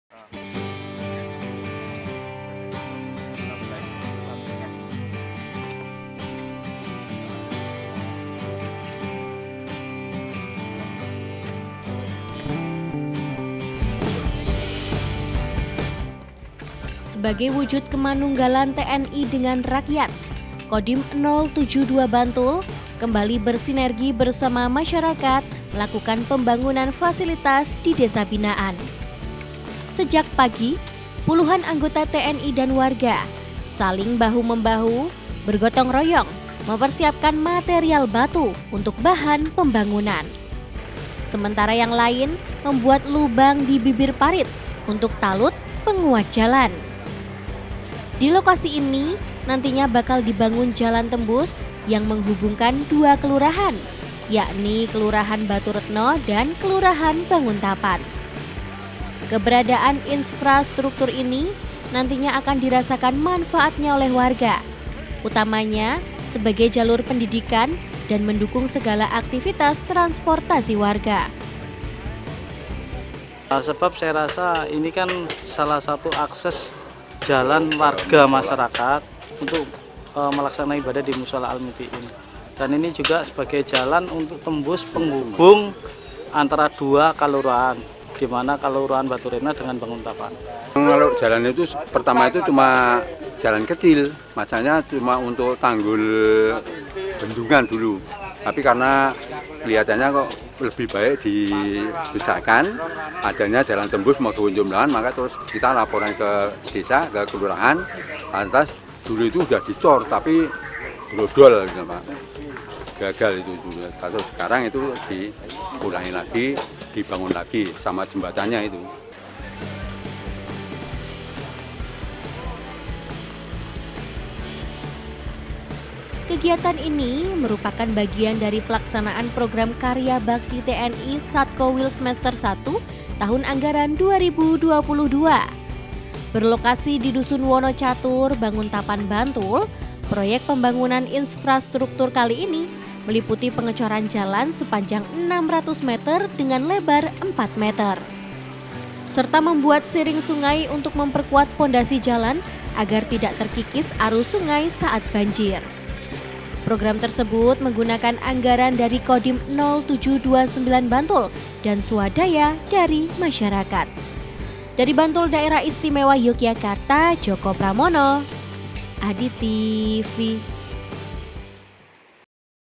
TNI DAN WARGA GOTONG ROYONG BANGUN JALAN TEMBUS PENGHUBUNG DESA
Kodim Nol Tujuh Dua Sembilan Bantul, kembali mewujudkan kemanunggalannya, bersama masyarakat, dalam program karya bakti T-N-I. Kali ini, sasarannya adalah pembuatan jalan tembus penghubung desa, di Dusun Wonocatur,  Banguntapan, Bantul, guna mendukung aktivitas perekonomian masyarakat, dan jalur pendidikan.